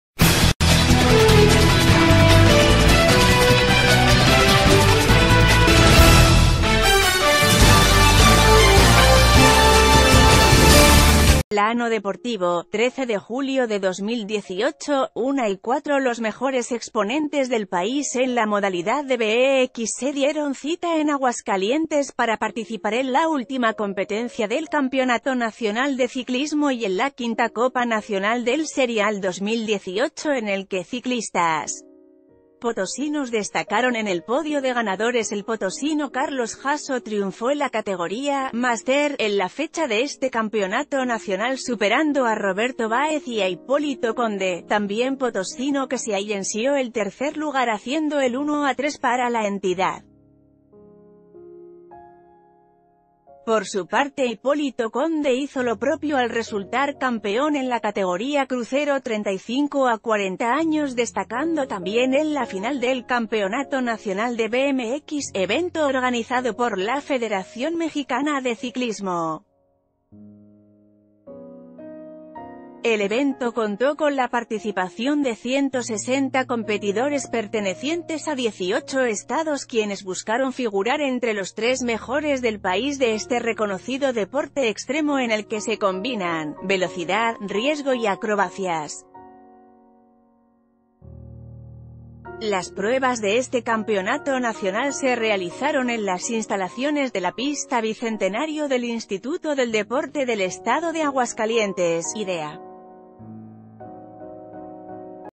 Logran potosinos campeonato en Nacional de BMX
Plano Deportivo | 13 Julio 2018 | 13:04 Los mejores exponentes del país en la modalidad de BMX se dieron cita en Aguascalientes para participar en la última competencia del Campeonato Nacional de Ciclismo y en la 5ª Copa Nacional del Serial 2018 en el que ciclistas potosinos destacaron en el podio d...